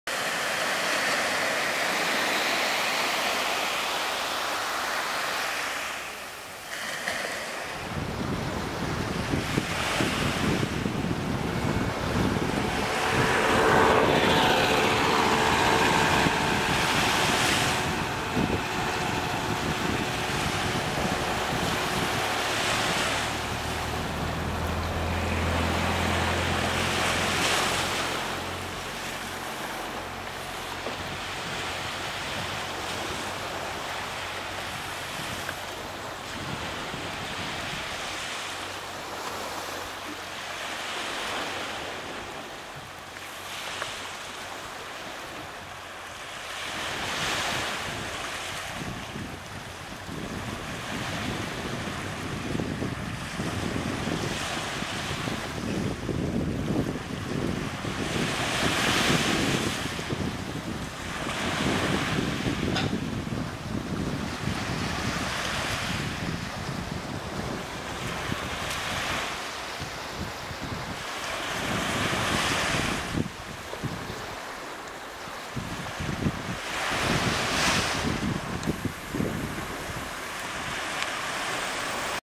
Andros Town
Views of Andros Town, the Chora of Andros island Greece, as seen from Chora beach on the north side of town. The video was shot on a cloudy morning one of the three days we spent in Andros Town in late May 2015.